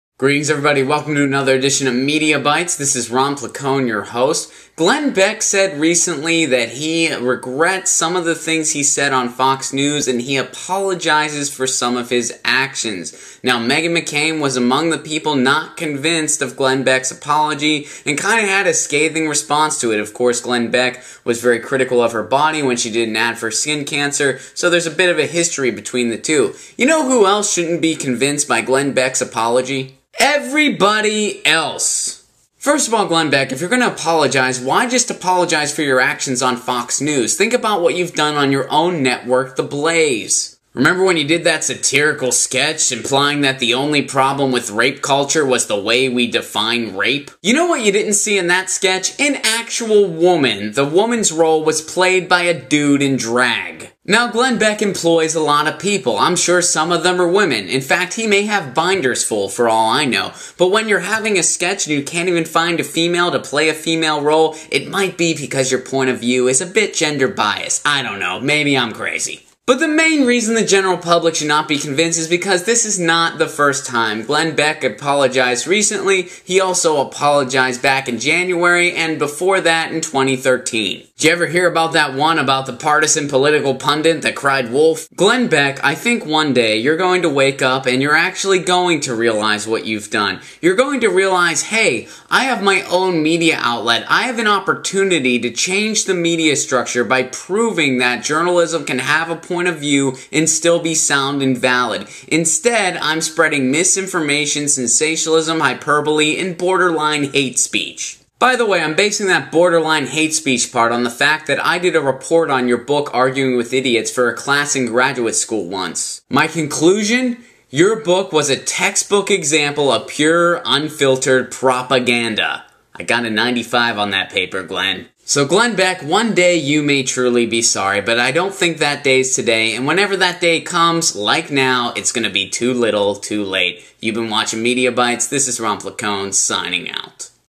Why Pundit’s Apology is BOGUS!
Glenn Beck recently apologized for his actions on Fox News implying he helped divide the United States. This is not his first time issuing an apology like this. Meghan McCain is not convinced by this apology, nobody else should be either. This MediaBytes clip features Ron Placone.